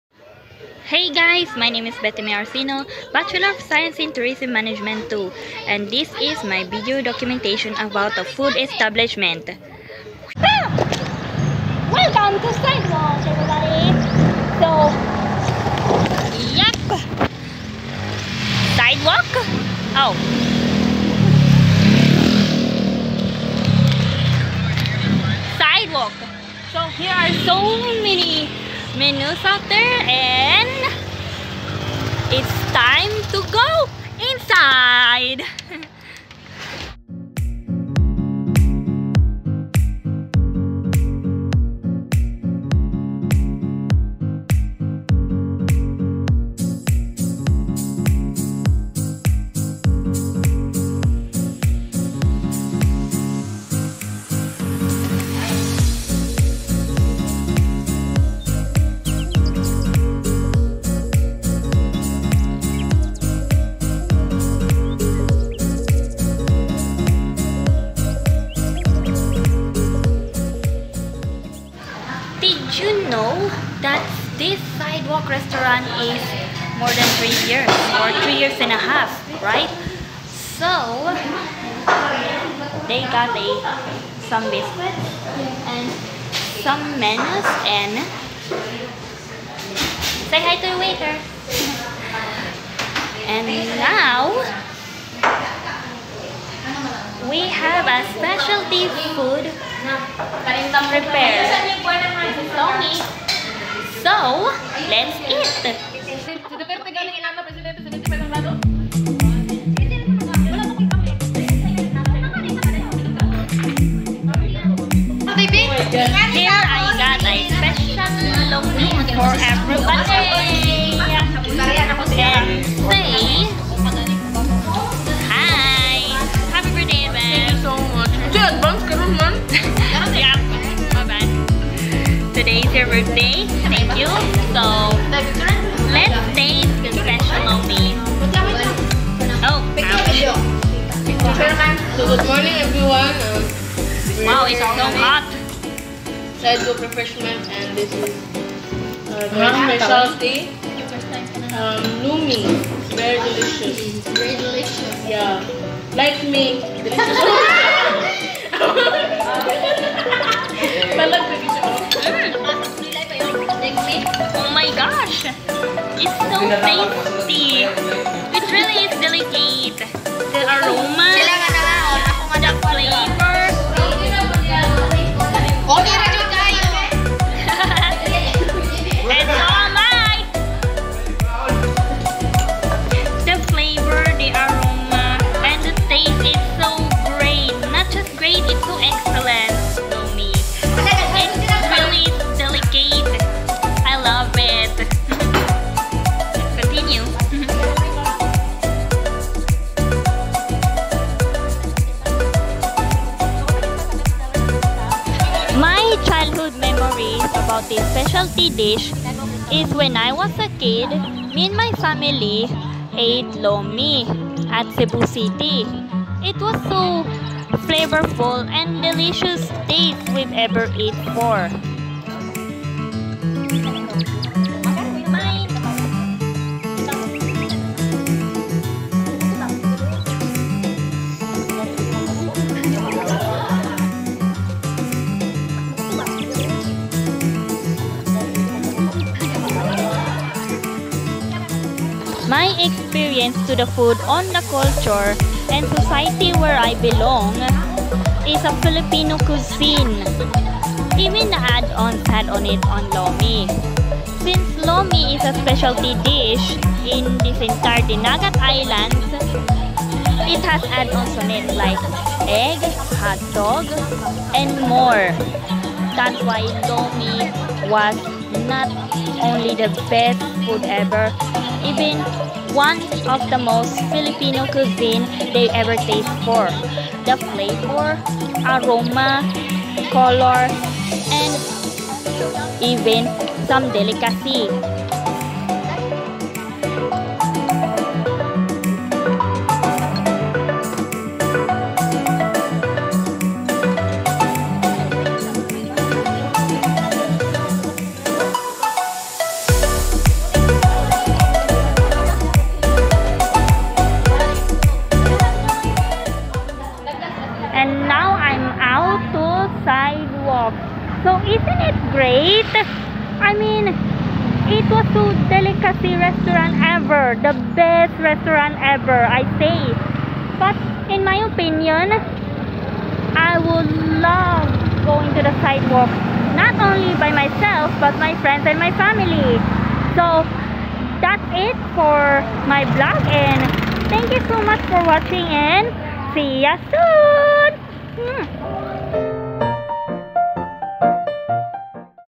Betty Mae's 2nd Vlog: 3-year Food Establishment at Sidewalk (GASTRONOMY: Food and Culture)
11/22/2022
Hey, guys! This is my 2nd Vlog that which related to our course "GASTRONOMY (Food & Culture)". Yesterday, me and my friends were going to a 3-year food establishment which is Sidewalk Food Hauz at Don Ruben, San Jose, Dinagat Islands to order a specialty food called "Lomi" with Coca-cola drinks. Plus, I'll observe the taste, aroma, and more delicacy about Lomi.

Actually, I'm not good at editing so please comment your reaction of my video and I'll try my best to do it! See ya soon! 👋